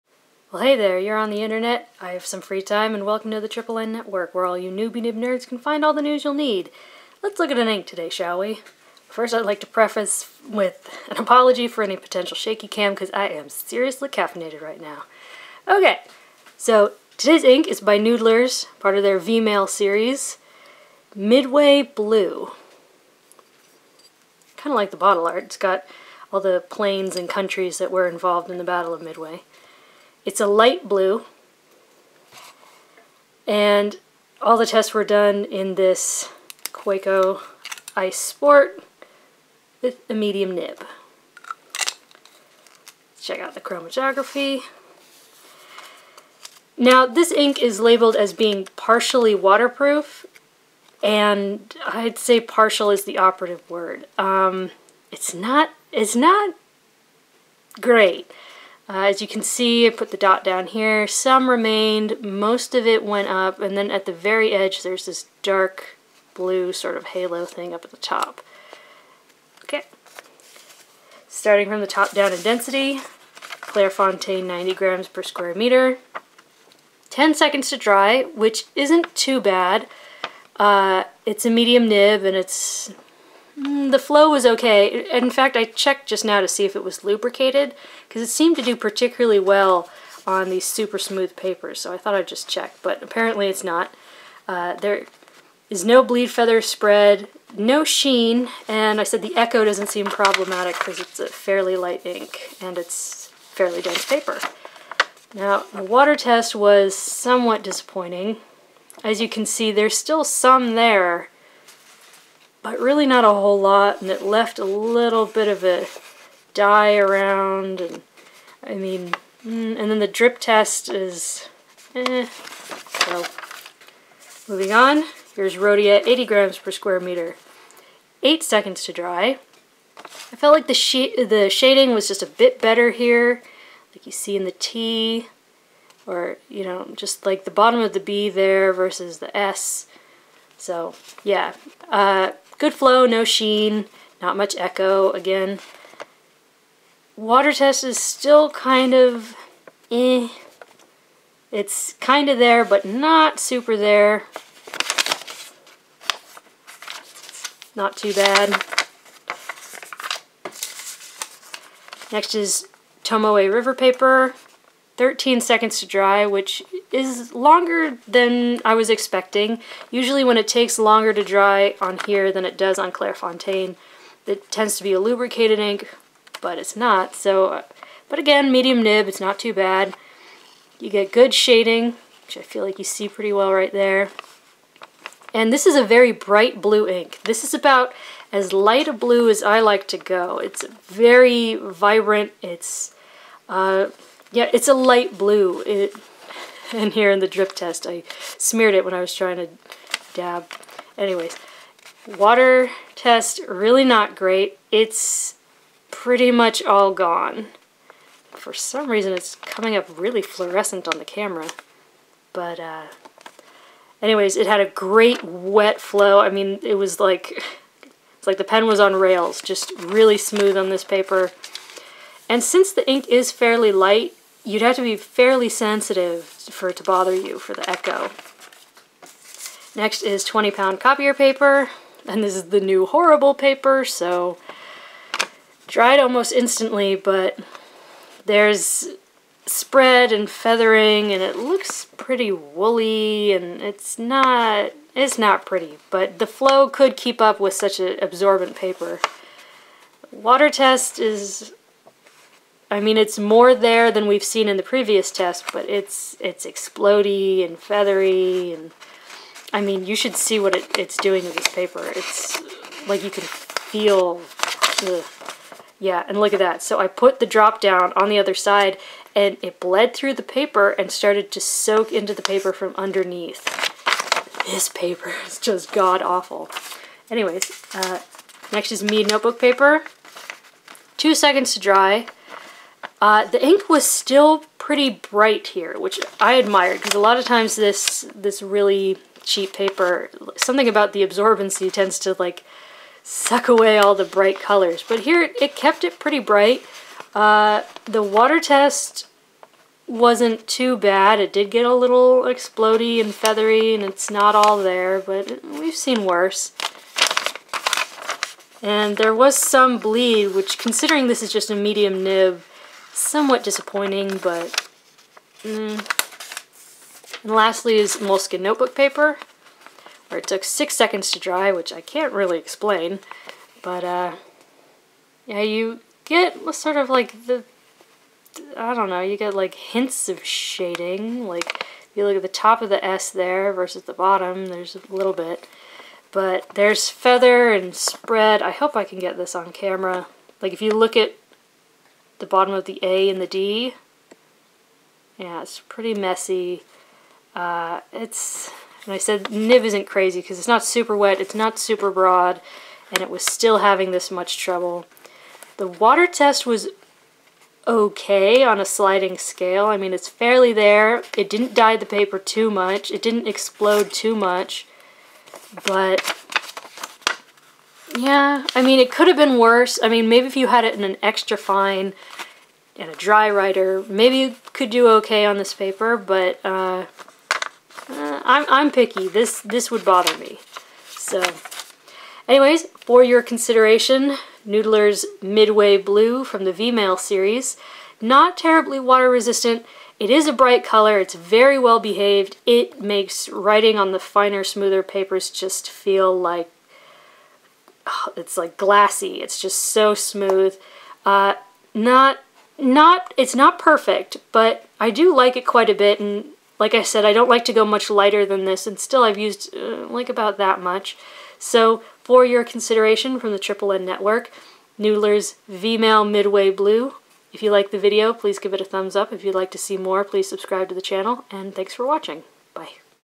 Ink Review: Noodler's Midway Blue Ink
Here I review an ink by Noodler's Ink, this time from their V-Mail Series by the title of "Midway Blue". Here-in may be found my own thoughts and opinions on the matter, as well as some test results I have for (among other factors) water-resistance on Clairefontaine, Rhodia, Copier Paper, Mead Notebook Paper, Tomoe River Paper, and Moleskine papers, and using a Kaweco Ice-Sport, with a Medium nib as a pen for these tests/experiments.